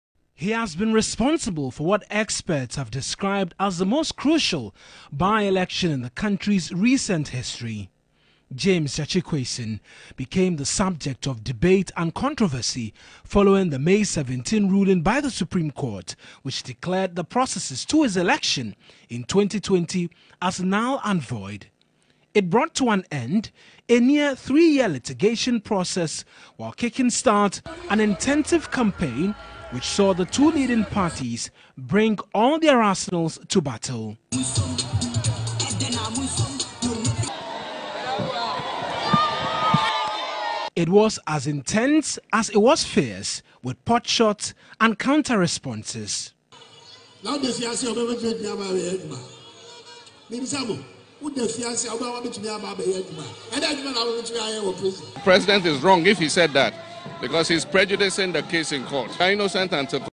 Assin North by-election: Parliament to swear in Quayson July 4.
Barring any glitches, James Gyakye Quayson will be sworn in on July 4 as new member of parliament for Assin North.
His return would however be the second time he is being sworn in parliament after his initial election was annulled by the Supreme Court.